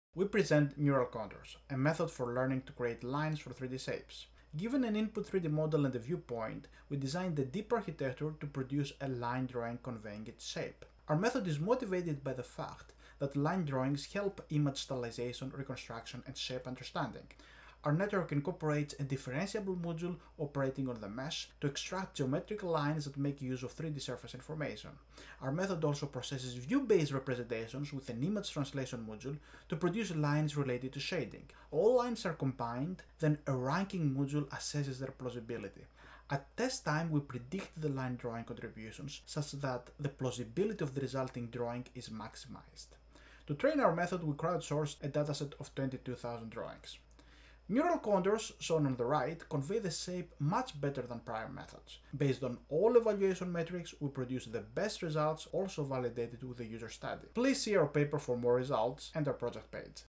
Neural Contours: Learning to Draw Lines from 3D Shapes
A short 1-min overview of our CVPR 2020 paper:

Difan Liu, Mohamed Nabail, Evangelos Kalogerakis, Aaron Hertzmann
Proceedings of the Computer Vision and Pattern Recognition (CVPR) 2020

This paper introduces a method for learning to generate line drawings from 3D models. Our architecture incorporates a differentiable module operating on geometric features of the 3D model, and an image-based module operating on view-based shape representations. At test time, geometric and view-based reasoning are combined by a neural ranking module to create a line drawing. The model is trained on a large number of crowdsourced comparisons of line drawings. Experiments demonstrate that our method achieves significant improvements in line drawing over the state-of-the-art when evaluated on standard benchmarks, resulting in drawings that are comparable to those produced by experienced human artists.